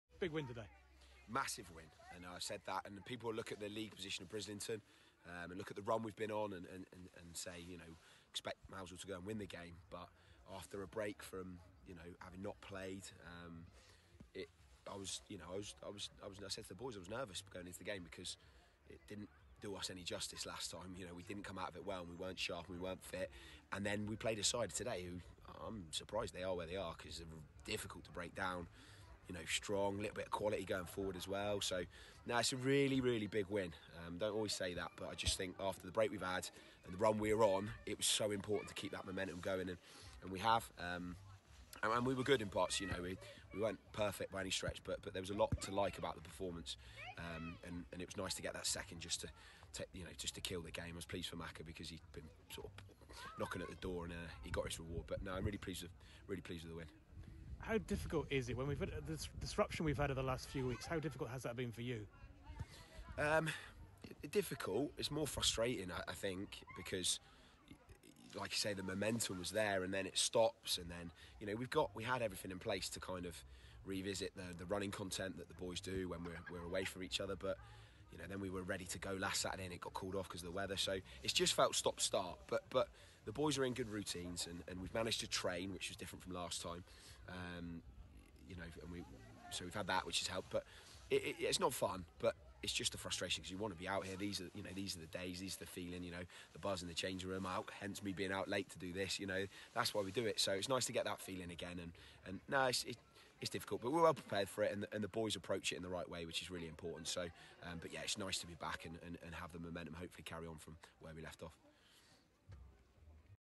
Mousehole 2-0 Brislington | Manager Jake Ash | Post match interview
Jake Ash speaks after The Seagulls 2-0 win over Brislington. Saturday 15th January 2022.